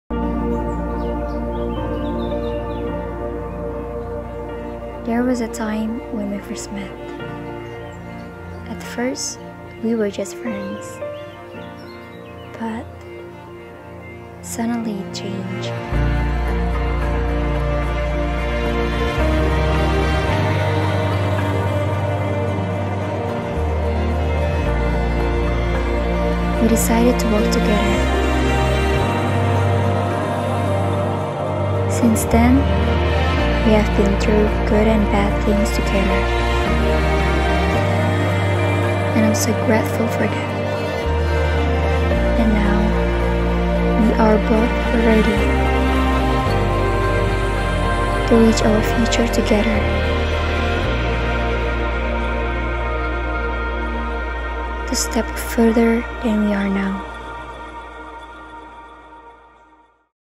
The love story of Eggy and Afina I
The preweding story by my recent project: drawbook.id
Will provide your love story!!

Gear i use:
Sony FE 28mm f2